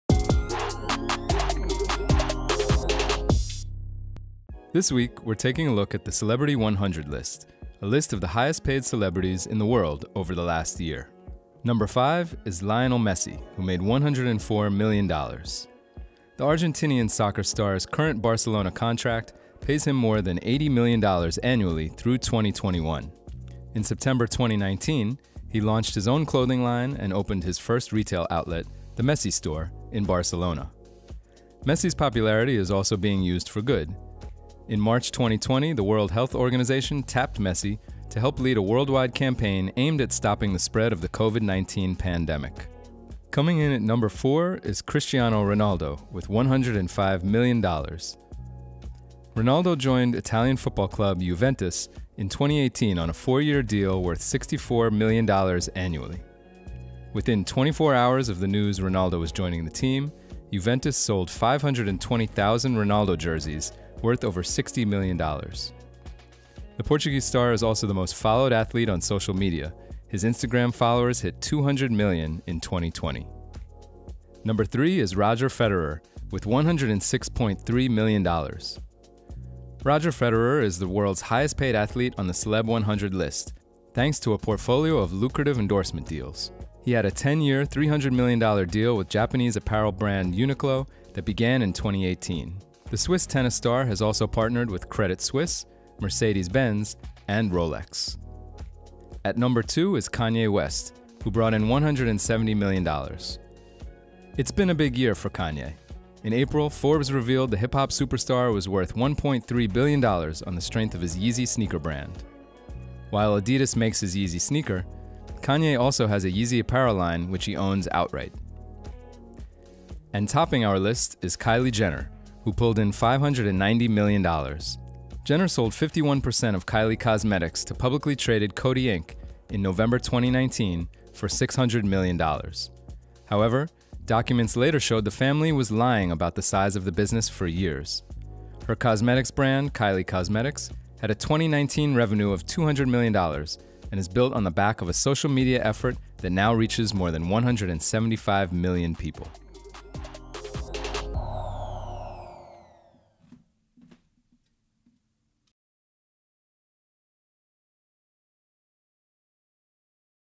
Top 5 Highest-Paid Celebrities 2020 | The Countdown | Forbes
The world’s highest-paid celebrities earned a combined $6.1 billion before taxes and fees, a $200 million drop from 2019, after the global pandemic shuttered stadiums and silenced performances around the world.

The top-earning celebrities were Kylie Jenner and Kanye West , who brought in $590 million and $170 million, respectively. West collected most of his earnings from his Yeezy sneakers deal with Adidas, while Jenner’s payday came from selling a 51% stake in her cosmetics firm to Coty in January. While she had exaggerated over the years about the size of her business, the money she pulled in from the deal was real--enough to rank as one of the biggest celebrity cash outs of all time. In sports, Roger Federer (No. 3, $106.3 million) is the first tennis player to earn the top spot among athletes on the list, while soccer’s Cristiano Ronaldo (No. 4, $105 million) became the first team player to earn $1 billion during his career. Naomi Osaka set her own record at No. 90, bringing in $37 million income, the most money any female athlete has ever made in a single year.

The list ranks “front of the camera” stars around the globe using pretax earnings from June 2019 through May 2020 before deducting fees for managers, lawyers and agents. Figures are based on information from Nielsen Music/MRC Data, Pollstar, IMDB, NPD BookScan and ComScore as well as interviews with industry experts and many of the stars themselves.